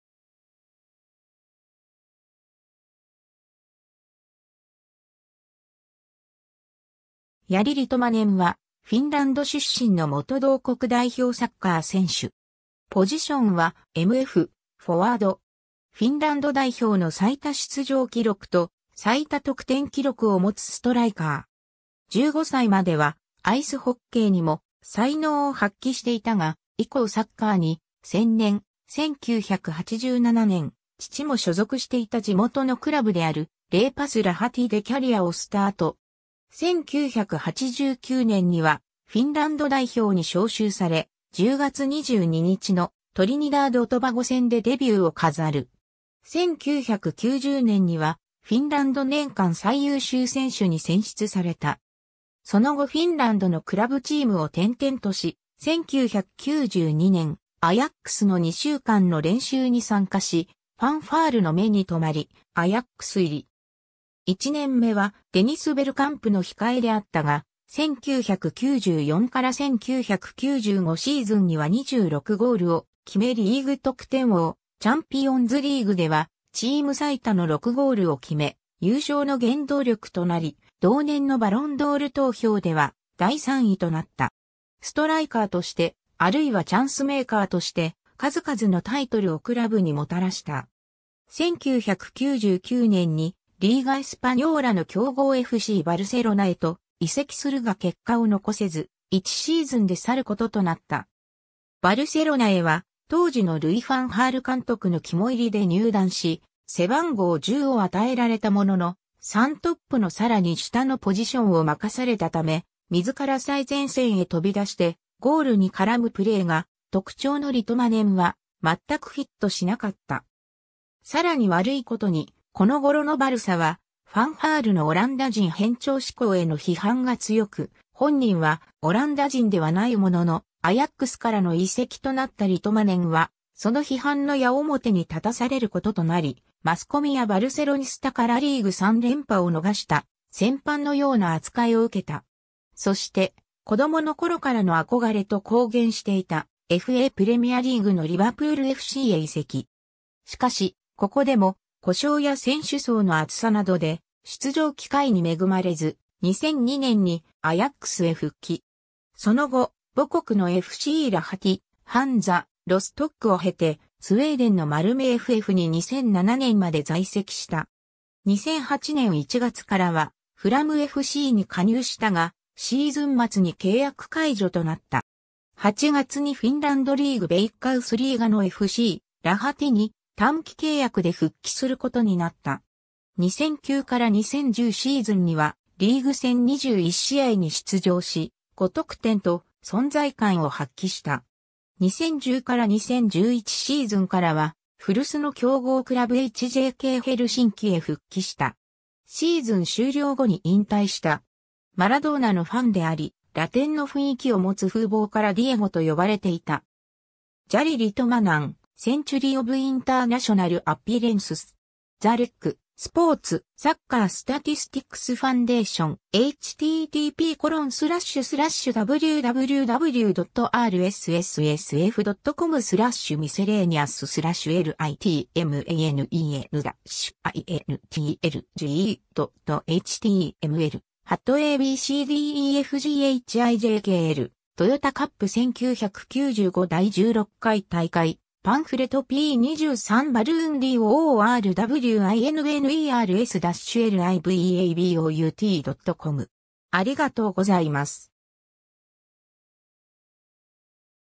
ヤリ・リトマネン
ハンザ・ロストックの選手
ヤリ・リトマネン（Jari Litmanen, 1971年2月20日 - ）は、フィンランド出身の元同国代表サッカー選手。
ポジションはMF、フォワード。
 フィンランド代表の最多出場記録と最多得点記録を持つストライカー。
 15歳まではアイスホッケーにも才能を発揮していたが、以降サッカーに専念、1987年、父も所属していた地元のクラブであるレイパス・ラハティでキャリアをスタート。
1989年にはフィンランド代表に招集され、10月22日のトリニダード・トバゴ戦でデビューを飾る。
1990年にはフィンランド年間最優秀選手に選出された。
 その後フィンランドのクラブチームを転々とし、1992年アヤックスの2週間の練習に参加し、ファンファールの目に留まり、アヤックス入り。
1年目はデニス・ベルカンプの控えであったが、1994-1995シーズンには26ゴールを決めリーグ得点王、チャンピオンズリーグではチーム最多の6ゴールを決め、優勝の原動力となり、同年のバロンドール投票では第3位となった。
ストライカーとして、あるいはチャンスメーカーとして、数々のタイトルをクラブにもたらした。
 1999年にリーガ・エスパニョーラの強豪FCバルセロナへと移籍するが結果を残せず、1シーズンで去ることとなった。
 バルセロナへは当時のルイ・ファン・ハール監督の肝煎りで入団し、背番号10を与えられたものの、3トップの更に下のポジションを任されたため、自ら最前線へ飛び出してゴールに絡むプレーが特徴のリトマネンは全くフィットしなかった。
 更に悪いことに、この頃のバルサはファン・ハールのオランダ人偏重志向への批判が強く、本人はオランダ人ではないものの、アヤックスからの移籍となったリトマネンは、その批判の矢面に立たされることとなり、マスコミやバルセロニスタからリーグ3連覇を逃した戦犯のような扱いを受けた。
 そして、子供の頃からの憧れと公言していたFAプレミアリーグのリヴァプールFCへ移籍。
しかし、ここでも故障や選手層の厚さなどで出場機会に恵まれず、2002年にアヤックスへ復帰。
その後、母国のFCラハティ、ハンザ・ロストックを経て、スウェーデンのマルメFFに2007年まで在籍した。
 2008年1月からはフラムFCに加入したが、シーズン末に契約解除となった。
8月にフィンランドリーグ・ヴェイッカウスリーガのFCラハティに短期契約で復帰することになった。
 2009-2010シーズンにはリーグ戦21試合に出場し、5得点と存在感を発揮した。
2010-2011シーズンからは古巣の強豪クラブHJKヘルシンキへ復帰した。
シーズン終了後に引退した。
 マラドーナのファンであり、ラテンの雰囲気を持つ風貌からディエゴと呼ばれていた。
^ “Jari Litmanen - Century of International Appearances” (英語). The Rec.Sport.Soccer Statistics Foundation. ^ a b c d e f g h i j k l 　TOYOTA CUP 1995　第16回大会パンフレト　p.23 ^ Ballon d’Or Winners-liveabout.com